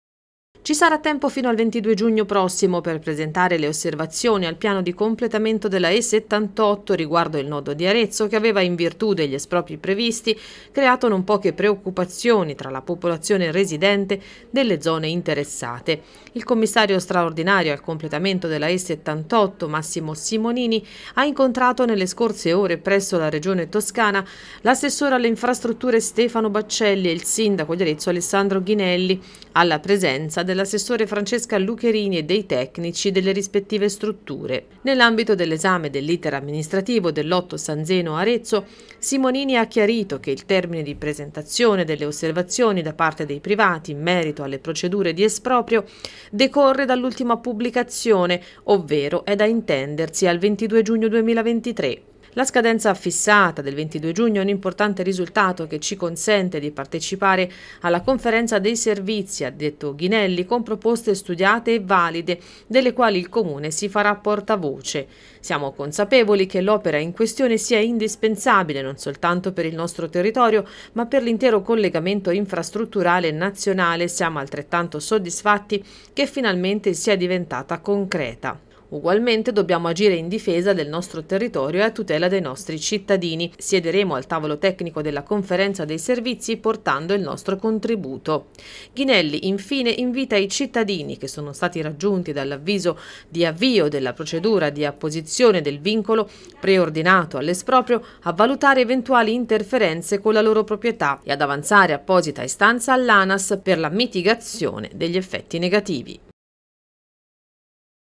E78, per il nodo di Arezzo c'è tempo fino al 22 giugno per le osservazioni
Ci sarà tempo fino al 22 giugno prossimo per presentare le osservazioni al piano di completamento della E78 riguardo il nodo di Arezzo, che aveva, in virtù degli espropri previsti, creato non poche preoccupazioni tra la popolazione residente nelle zone interessate.
Il commissario straordinario al completamento della E78 Massimo Simonini ha incontrato nelle scorse ore presso la Regione Toscana l’assessore alle infrastrutture Stefano Baccelli e il sindaco di Arezzo Alessandro Ghinelli, alla presenza dell'assessore Francesca Lucherini e dei tecnici delle rispettive strutture.
Nell’ambito dell’esame dell’iter amministrativo del lotto San Zeno-Arezzo, Simonini ha chiarito che il termine di presentazione delle osservazioni da parte dei privati, in merito alle procedure espropriative, decorre dall’ultima pubblicazione, avvenuta ieri, pertanto è da intendersi al 22 giugno 2023.
“La scadenza fissata al 22 giugno è un importante risultato che ci consente di partecipare alla conferenza dei servizi con proposte studiate e valide, delle quali il Comune si farà portavoce nell'interesse dei cittadini – ha commentato il sindaco Alessandro Ghinelli. Siamo consapevoli che l'opera in questione sia indispensabile non soltanto per il nostro territorio ma per l'intero collegamento infrastrutturale nazionale, e siamo altrettanto soddisfatti che finalmente sia diventata concreta e prossima la sua realizzazione. Ugualmente dobbiamo agire in difesa del nostro territorio e a tutela dei nostri cittadini. Siederemo al tavolo tecnico della conferenza dei servizi portando il nostro contributo affinché venga individuata la migliore soluzione a salvaguardia degli interessi di tutti. Invito – conclude Ghinelli - i cittadini che sono stati raggiunti dall’avviso di avvio della procedura di apposizione del vincolo preordinato all’esproprio, a valutare eventuali interferenze con le loro proprietà, e ad avanzare apposita istanza all’ANAS per la mitigazione degli effetti negativi”.
Nella zona di Arezzo il progetto prevede la realizzazione di un’infrastruttura stradale a 4 corsie nel tratto San Zeno – Arezzo - Palazzo del Pero, che consenta di dare continuità all’itinerario collegando i tratti già realizzati. L’intervento è diviso in due lotti.
Per il primo lotto, tra le località Santa Maria delle Grazie e Palazzo del Pero, di circa 8 km, è in corso la progettazione definitiva. In particolare, sono in fase di ultimazione le indagini geognostiche e di caratterizzazione ambientale. Per il secondo lotto, tra le località San Zeno e Santa Maria delle Grazie, di circa 5 km, sono in corso le procedure di legge per l’approvazione del progetto definitivo.